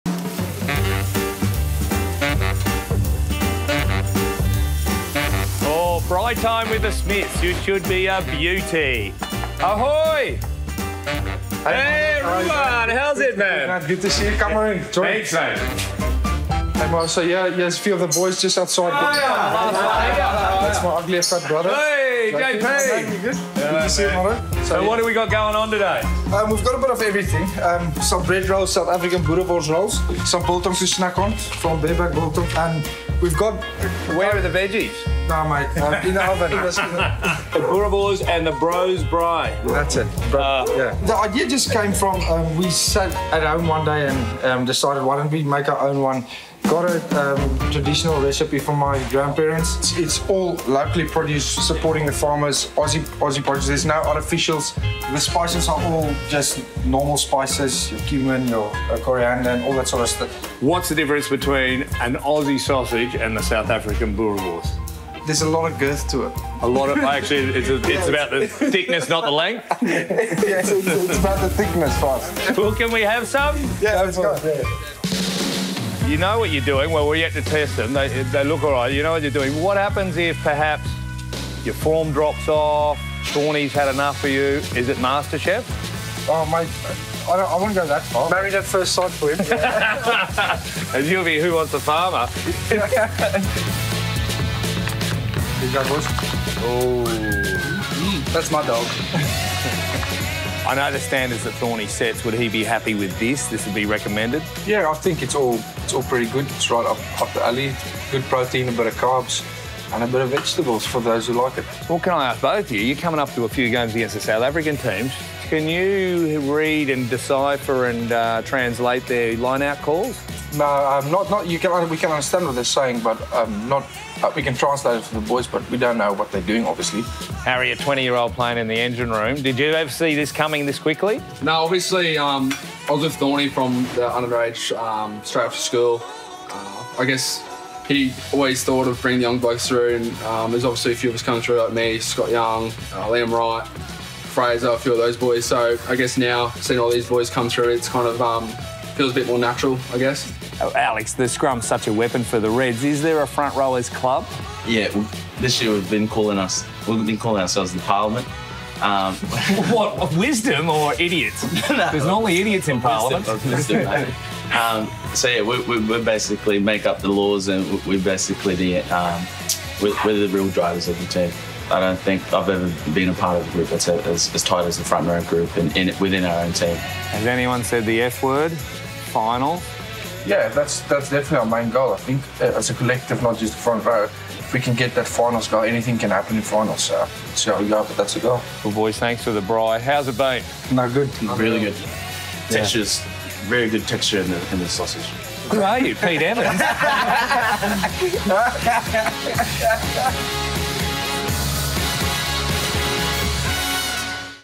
Super Rugby Kick Off: JP and Ruan Smith
Greg Martin visits the home of Queensland Reds props and twins JP and Ruan Smith as they chat about their new business venture.